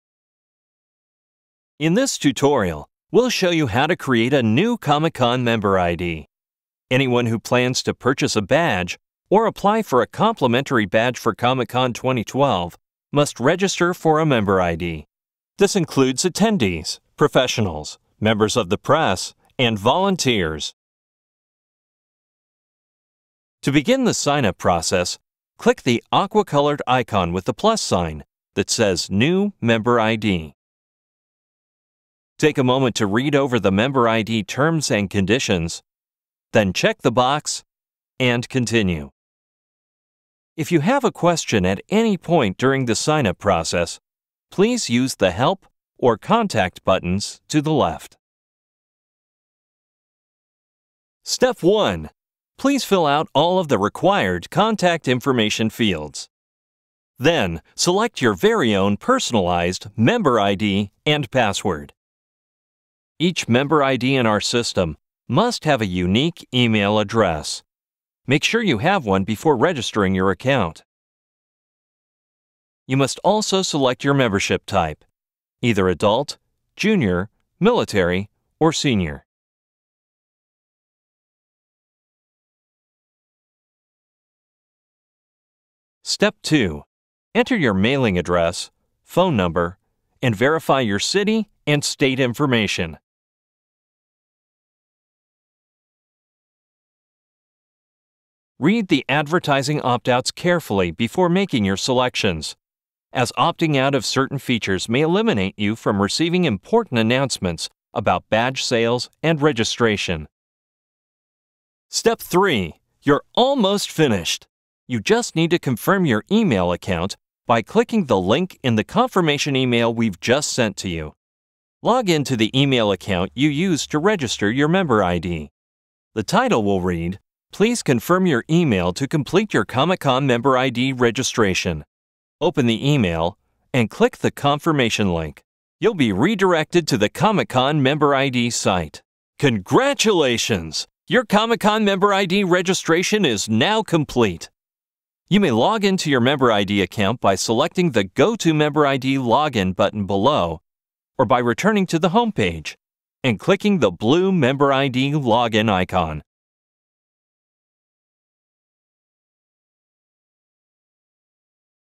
How to Create a New Comic-Con Member ID (no music)
Comic-Con Member ID Tutorial - Short Video on How to Create a New Member ID
(without music)